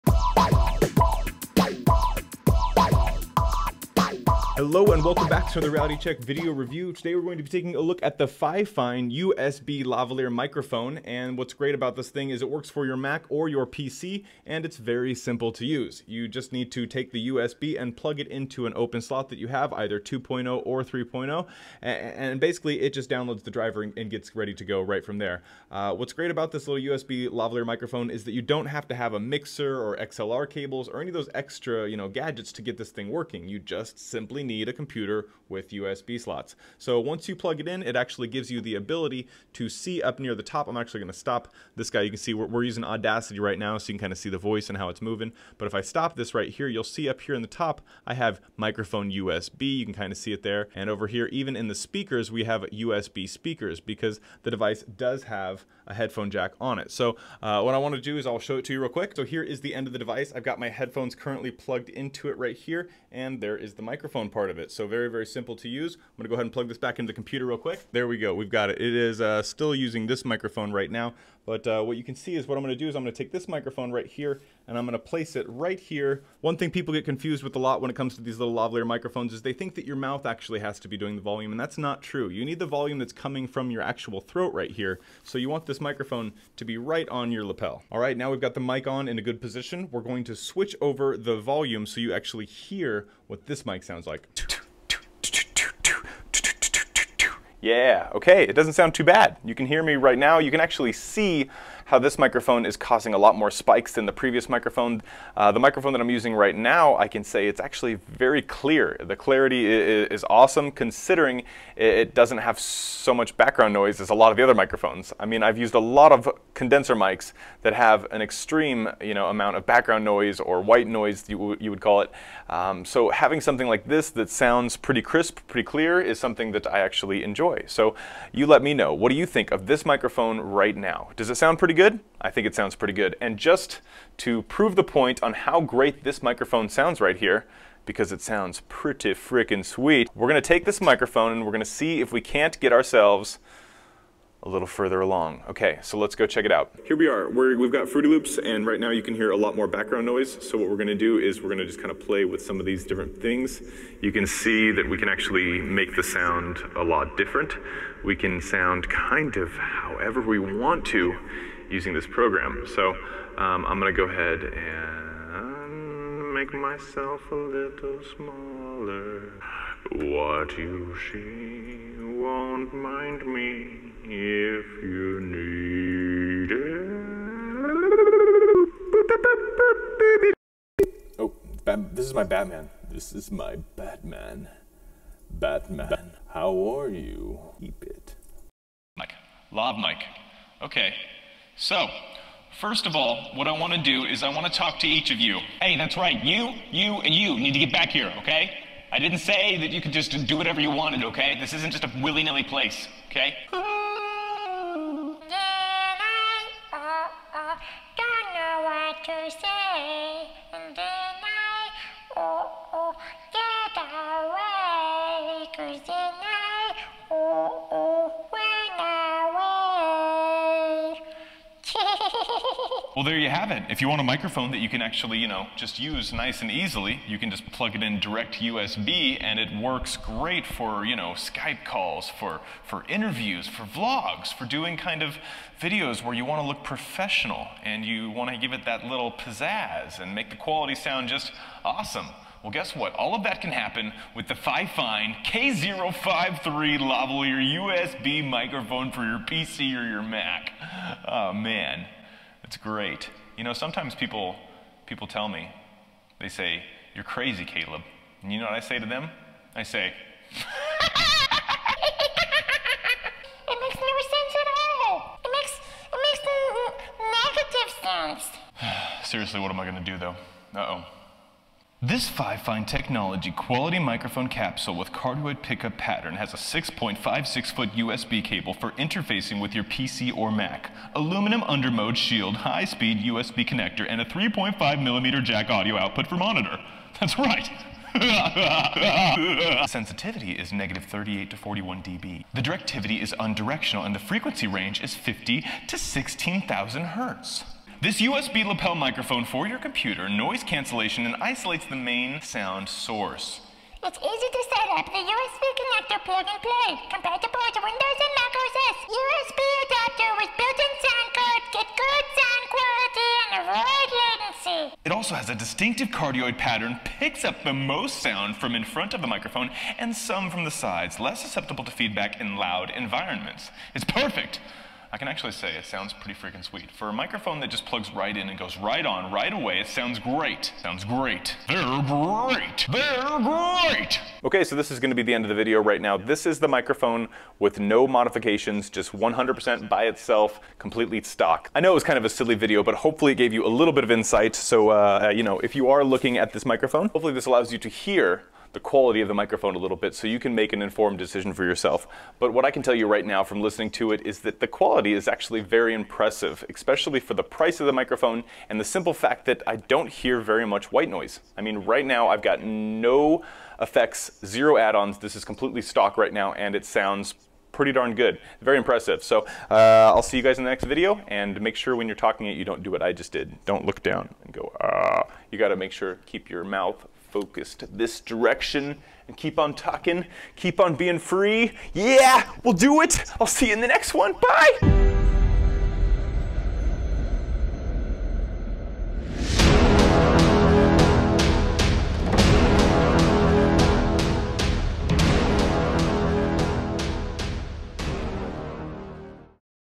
Plug & Play $18 USB Lavalier Microphone Review! | FIFINE K053
Here we take a quick look at the Fifine K053 Lavalier Lapel Clip-On USB Microphone. Raw audio tests in combination with audio filters from Fruity Loops. Quick Review and Unbox of the $18.00 Microphone.

►My TOP Amazon Picks

Some Cool Deals You Might Like!

$399 Limited Time OCULUS RIFT + TOUCH BUNDLE!!!